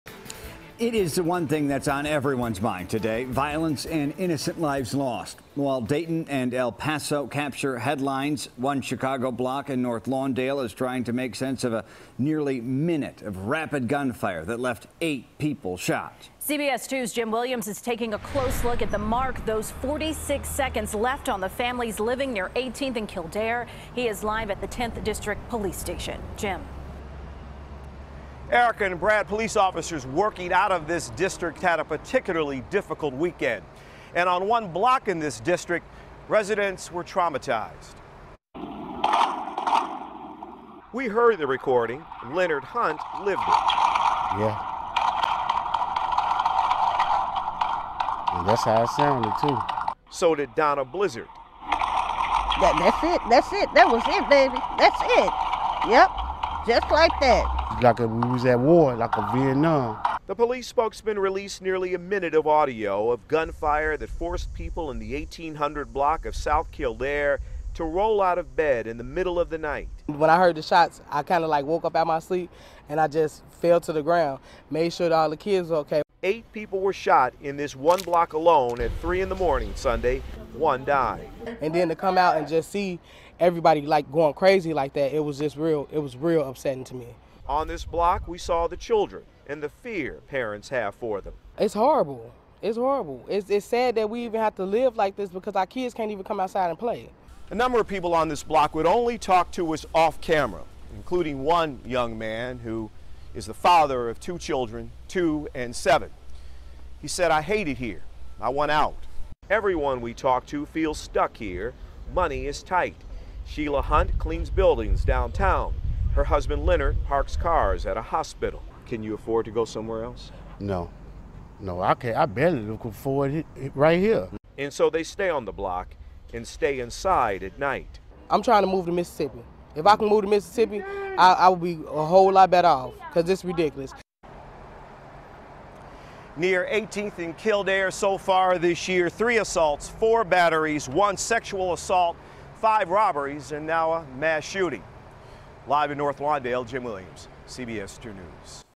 Lawndale Neighbors Beside Themselves After Shooting
Neighbors at 18th Street and Kildare Avenue are in fear and ire, after a mass shooting left one person dead and seven wounded on their block. CBS 2's Jim Williams reports.